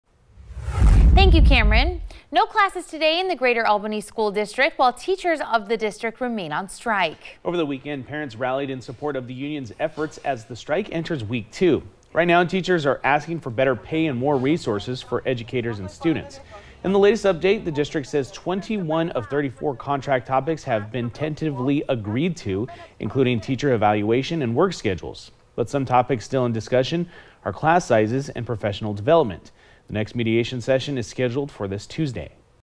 No class in Albany schools as strike continues
Class was cancelled on Monday and Tuesday, and will likely be cancelled as long as the strike continues.